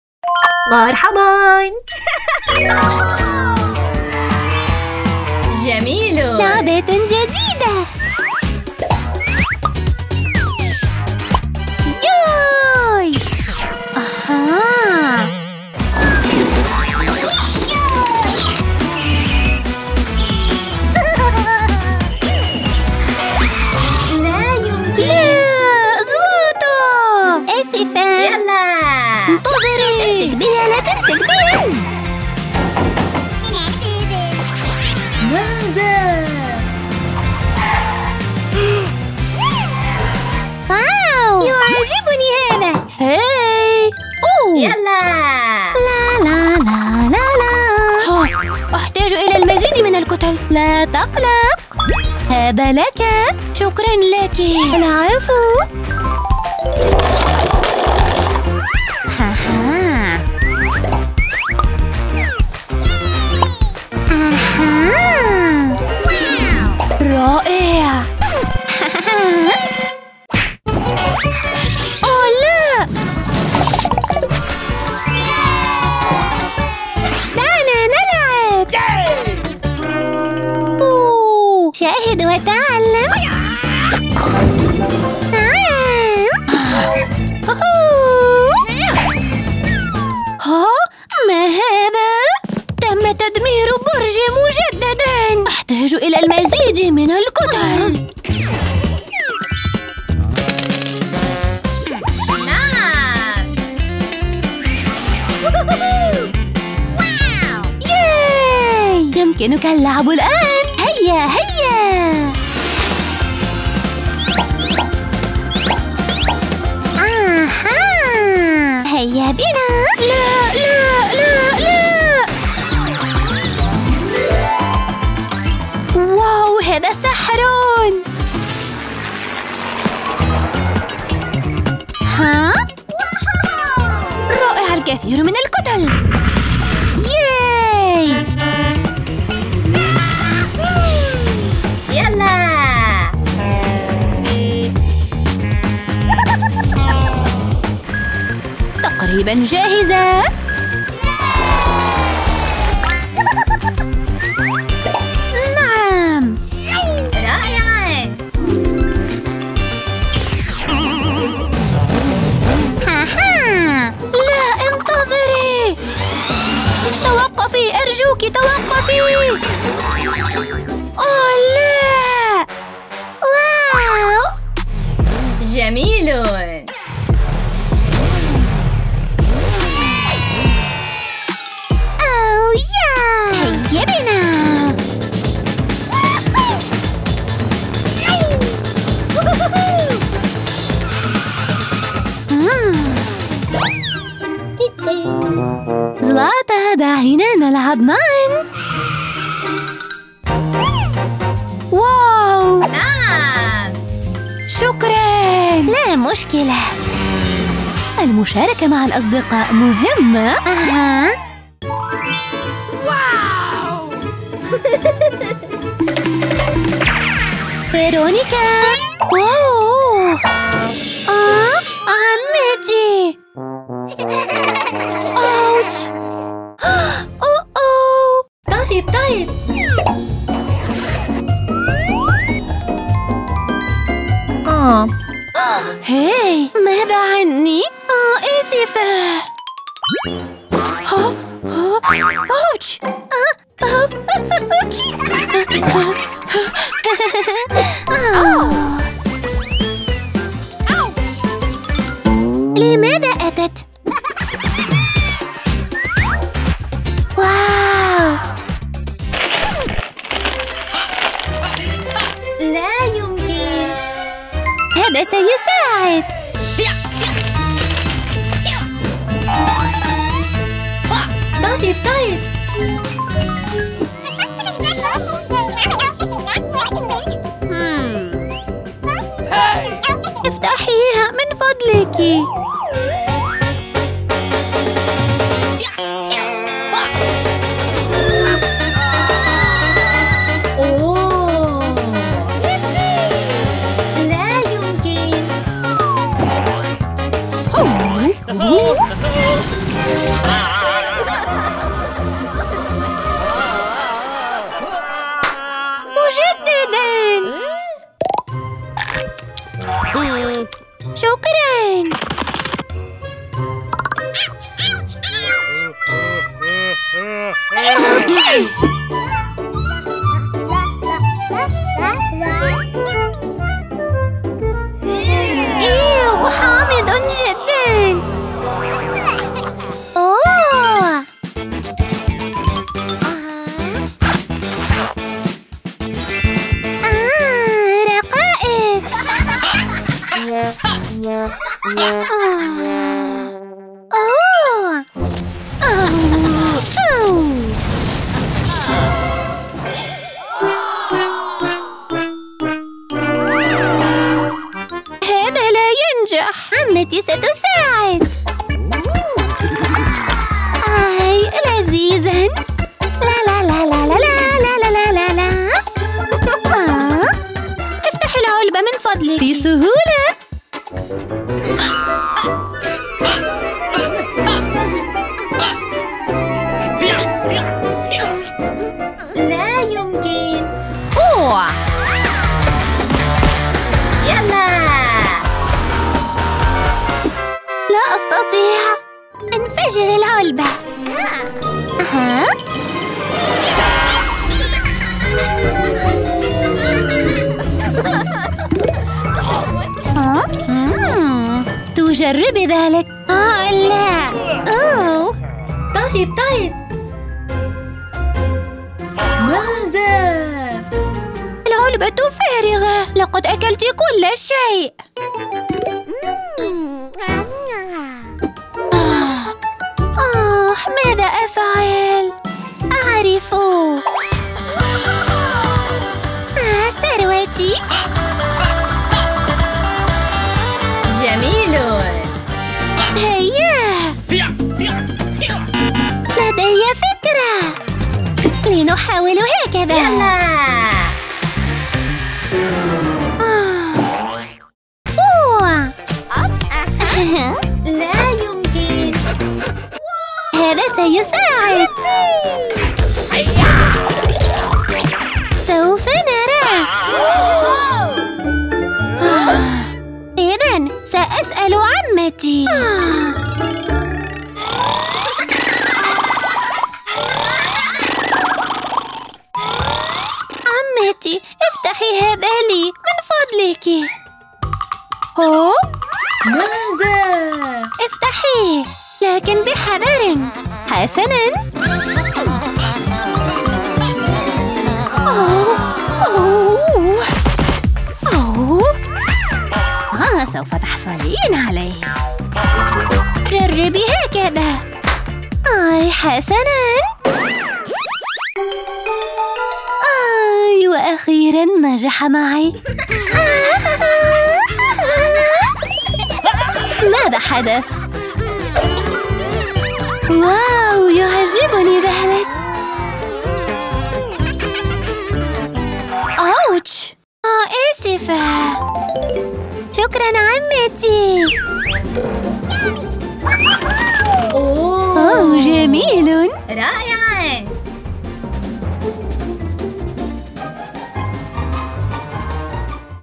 زلاتا وأصدقاؤه يلعبون بالألعاب
زلاتا والأصدقاء يشاركون ويلعبون بالألعاب في ألعاب الأطفال